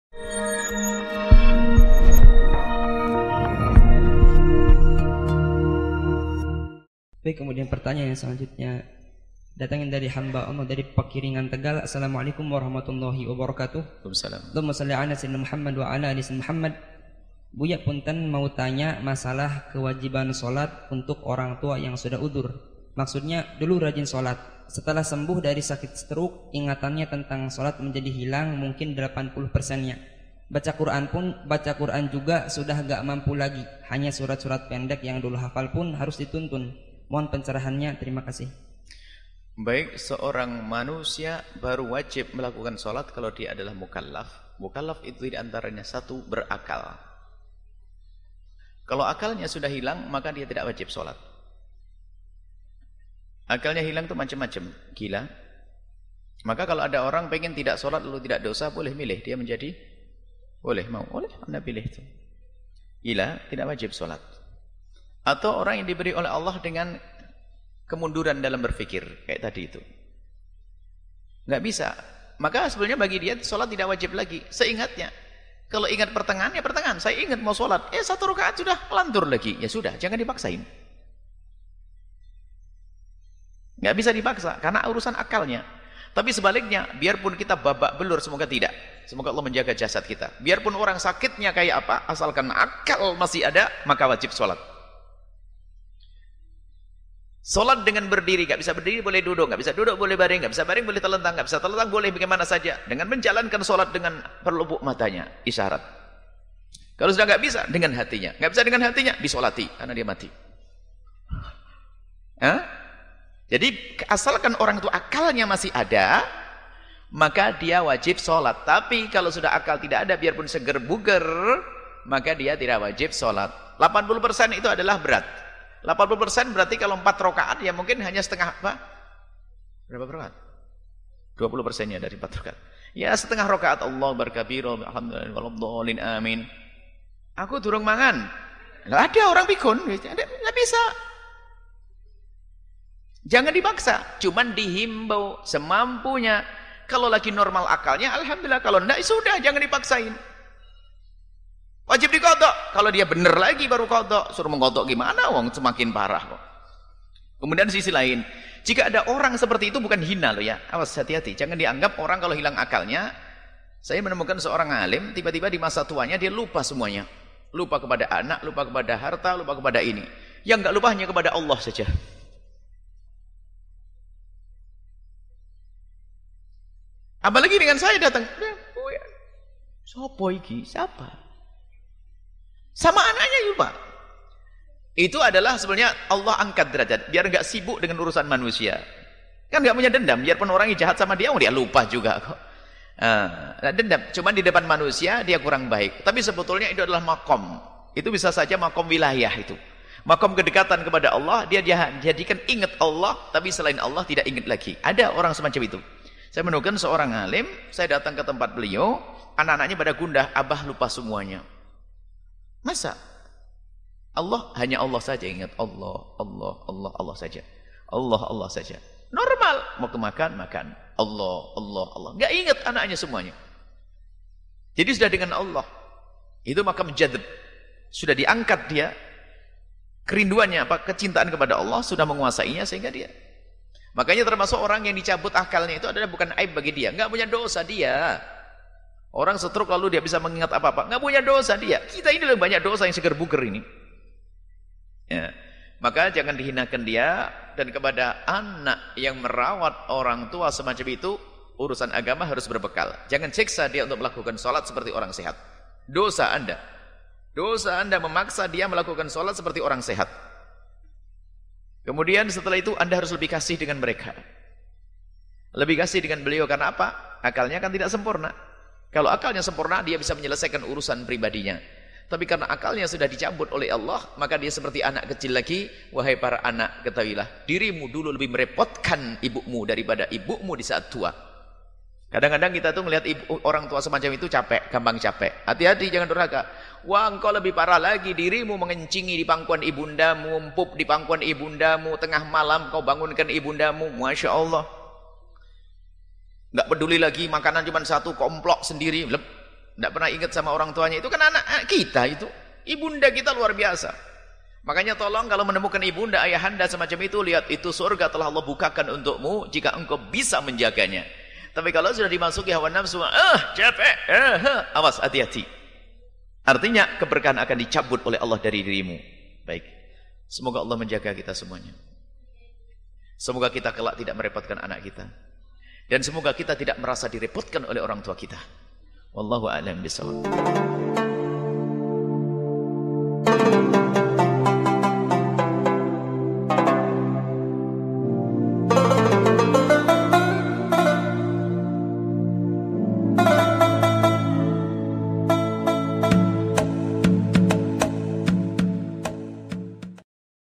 #148 Kewajiban sholat bagi yang hilang akal | Buya Yahya Menjawab
Bagi seseorang yang hilang akal, gila, lupa apakah tetap wajib sholat?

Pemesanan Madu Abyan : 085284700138
Line : @radioqu_cirebon